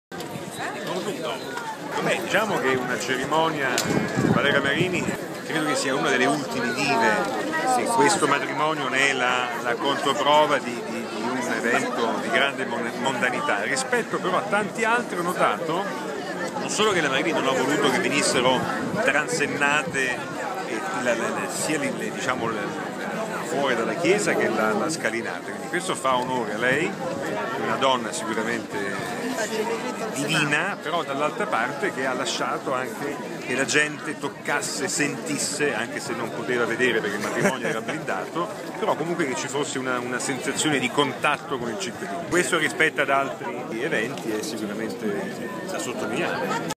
Gabriele Paolini al matrimonio di Valeria Marini
L'opinione di Gabriele Paolini sul matrimonio di Valeria Marini e Gianni Cottone